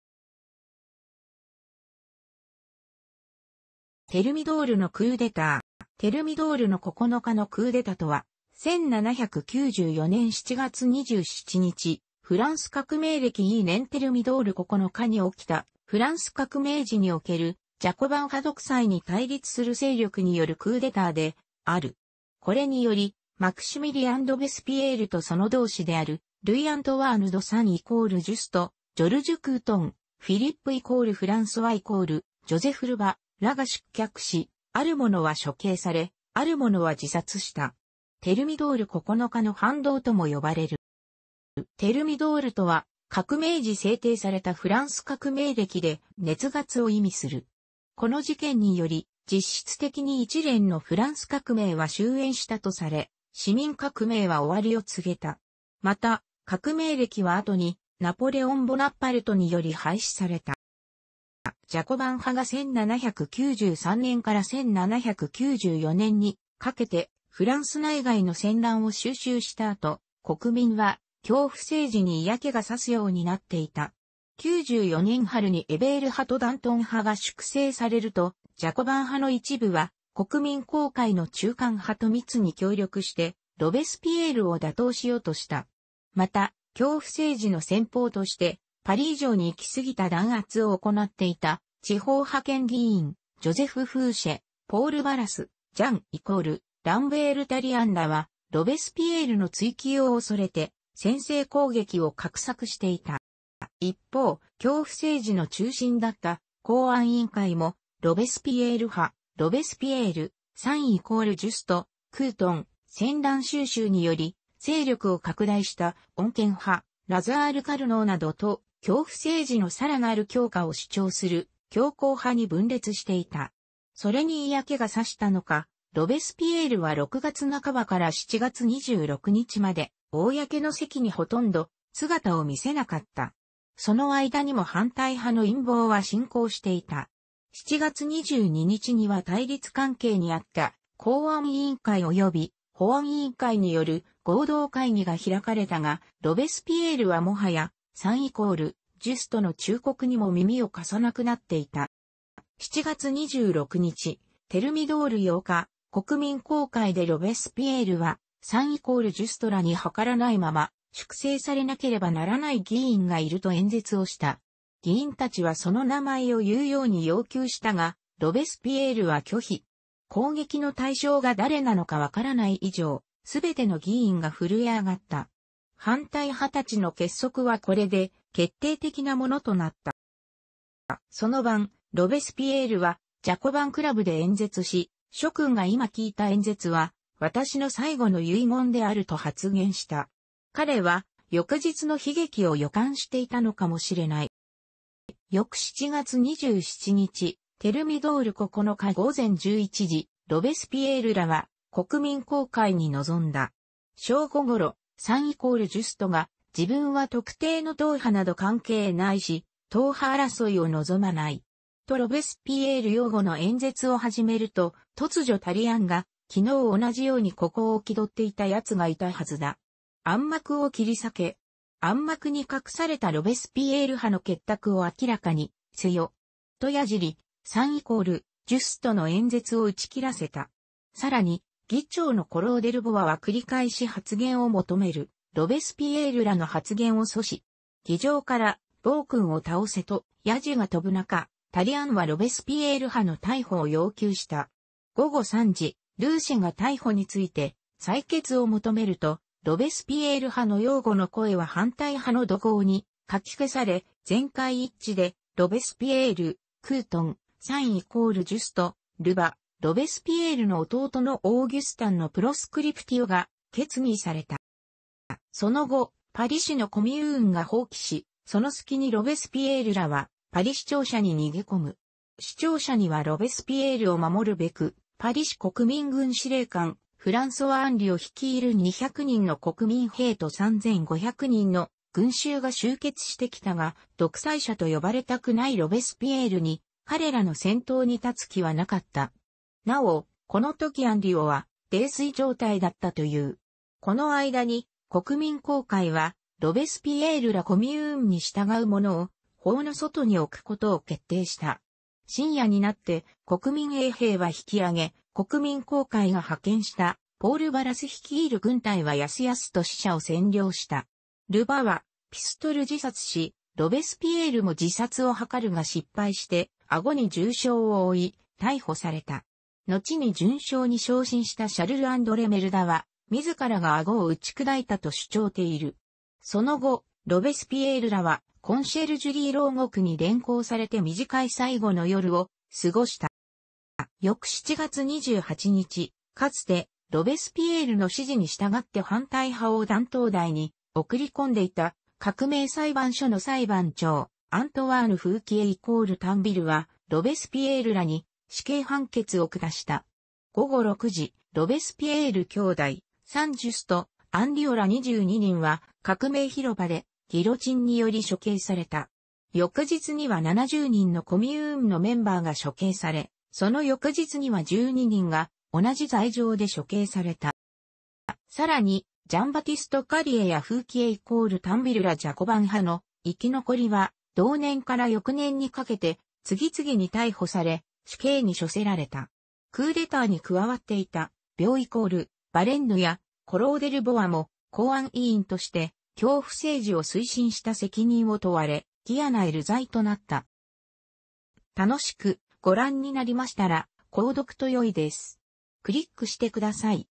テルミドールのクーデター
テルミドールの9日のクーデタとは、1794年7月27日（フランス革命暦II年テルミドール9日）に起きた、フランス革命時におけるジャコバン派独裁に対立する勢力によるクーデターである。これによりマクシミリアン・ロベスピエールとその同志であるルイ・アントワーヌ・ド・サン＝ジュスト、ジョルジュ・クートン、フィリップ＝フランソワ＝ジョゼフ・ル・バ（）らが失脚し、ある者は処刑され、ある者は自殺した。テルミドール9日の反動とも呼ばれる。

テルミドールとは、革命時制定されたフランス革命暦で「熱月」を意味する。この事件により実質的に一連のフランス革命は終焉したとされ、市民革命は終わりを告げた。又、革命暦は後にナポレオン・ボナパルトにより廃止された。

ジャコバン派が1793年から1794年にかけてフランス内外の戦乱を収拾した後、国民は恐怖政治に嫌気が差すようになっていた。94年春にエベール派とダントン派が粛清されると、ジャコバン派の一部は国民公会の中間派と密に協力してロベスピエールを打倒しようとした。また、恐怖政治の先鋒としてパリ以上に行き過ぎた弾圧を行っていた地方派遣議員（ジョゼフ・フーシェ、ポール・バラス、ジャン＝ランベール・タリアンら）は、ロベスピエールの追及を恐れて先制攻撃を画策していた。

一方、恐怖政治の中心だった公安委員会も、ロベスピエール派（ロベスピエール、サン＝ジュスト、クートン）・戦乱収拾により勢力を拡大した穏健派（ラザール・カルノーなど）と、恐怖政治のさらなる強化を主張する強硬派(ジャック・ニコラ・ビョー＝ヴァレンヌ、ジャン＝マリー・コロー・デルボワなど)に分裂していた。それに嫌気が差したのか、ロベスピエールは6月半ばから7月26日まで、公の席にほとんど姿を見せなかった。その間にも反対派の陰謀は進行していた。7月22日には対立関係にあった公安委員会および保安委員会による合同会議が開かれたが、ロベスピエールはもはやサン＝ジュストの忠告にも耳を貸さなくなっていた。

7月26日（テルミドール8日）、国民公会でロベスピエールは、サン＝ジュストらに諮らないまま「粛清されなければならない議員がいる」と演説をした。議員達はその名前を言うように要求したが、ロベスピエールは拒否。攻撃の対象が誰なのかわからない以上、全ての議員が震えあがった。反対派たちの結束はこれで決定的なものとなった。

その晩、ロベスピエールはジャコバン・クラブで演説し、｢諸君がいま聞いた演説は私の最後の遺言である｣と発言した。彼は翌日の悲劇を予感していたのかもしれない。

翌7月27日（テルミドール9日）午前11時、ロベスピエールらは国民公会に臨んだ。正午ごろ、サン＝ジュストが「自分は特定の党派など関係ないし、党派争いを望まない。」とロベスピエール擁護の演説を始めると、突如タリアンが「昨日同じように孤高を気取っていた奴がいたはずだ。暗幕を切り裂け。（暗幕に隠されたロベスピエール派の結託を明らかにせよ。）」と野次り、サン＝ジュストの演説を打ち切らせた。さらに議長のコロー・デルボワは繰り返し発言を求めるロベスピエールらの発言を阻止。議場から｢暴君を倒せ｣と野次が飛ぶなか、タリアンはロベスピエール派の逮捕を要求した。午後3時、ルーシェが逮捕について採決を求めると、ロベスピエール派の擁護の声は反対派の怒号にかき消され、全会一致でロベスピエール、クートン、サン＝ジュスト、ル・バ、ロベスピエールの弟のオーギュスタンのプロスクリプティオが決議された。

その後、パリ市のコミューンが蜂起し、そのすきにロベスピエールらはパリ市庁舎に逃げ込む。市庁舎にはロベスピエールを守るべくパリ市国民軍司令官フランソワ・アンリオ率いる200人の国民衛兵と3500人の群集が集結してきたが、独裁者と呼ばれたくないロベスピエールに彼らの先頭に立つ気はなかった。なお、このときアンリオは泥酔状態だったという。この間に、国民公会は、ロベスピエールらコミューンに従うものを法の外に置くことを決定した。深夜になって国民衛兵は引き上げ、国民公会が派遣したポール・バラス率いる軍隊はやすやすと市庁舎を占領した。ル・バはピストル自殺し、ロベスピエールも自殺を図るが失敗して顎に重傷を負い、逮捕された。のちに准将に昇進したシャルル・アンドレ・メルダは自らが顎を撃ち砕いたと主張している。その後、ロベスピエールらはコンシェル...